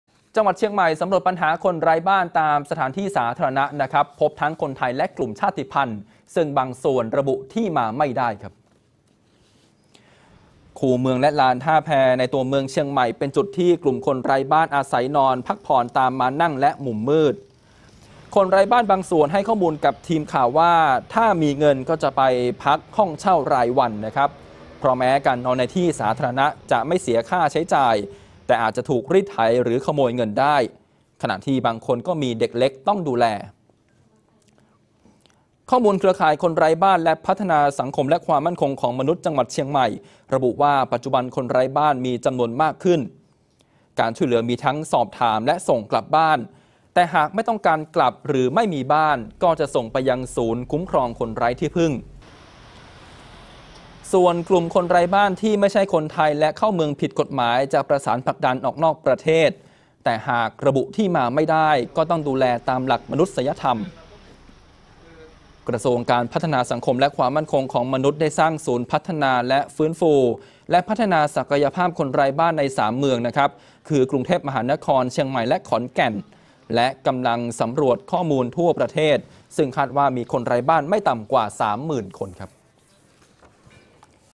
คนไร้บ้านเชียงใหม่
12-7-62
...การสำรวจข้อมูลคนไร้บ้านทั่วประเทศ โดยกระทรวงการพัฒนาสังคมและความมั่นคงของมนุษย์ ร่วมกันองค์กรเครือข่าย คาดว่าจะมีจำนวนไม่ต่ำกว่า 3 หมื่นคน โดยที่จังหวัดเชียงใหม่ มีการสำรวจพบคนไร้บ้านหน้าใหม่เพิ่มมากขึ้น โดยเฉพาะกลุ่มชาติพันธุ์
ข่าว/ภาพ...ทีมศูนย์ข่าวภาคเหนือ